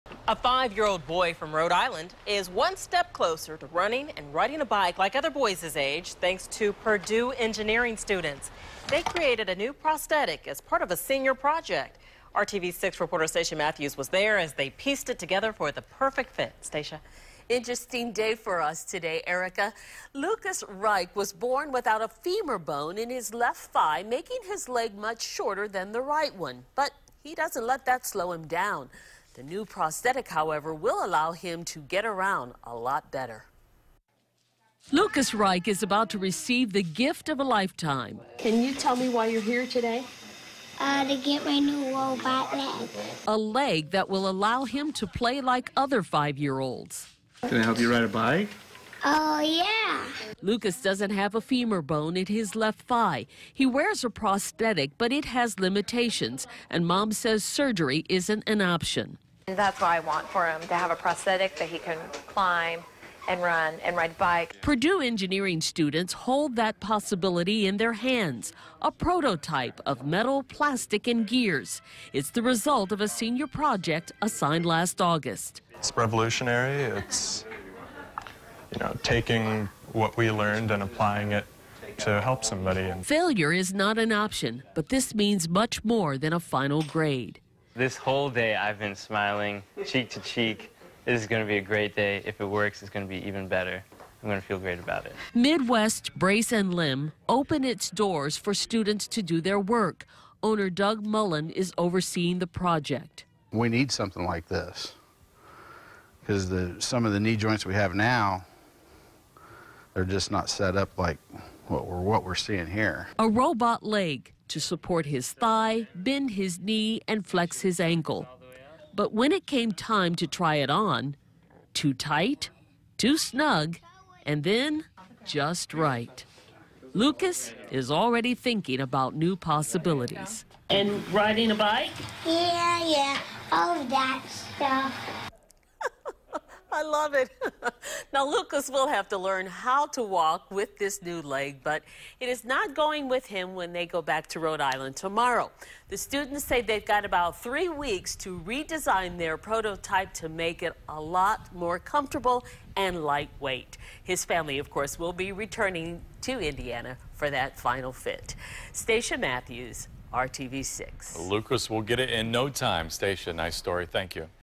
Purdue University students build innovative prosthetic leg for 5-year-old with rare birth defect
A team of Purdue University students has designed a new type of prosthetic leg for a 5-year-old boy born with a rare leg condition.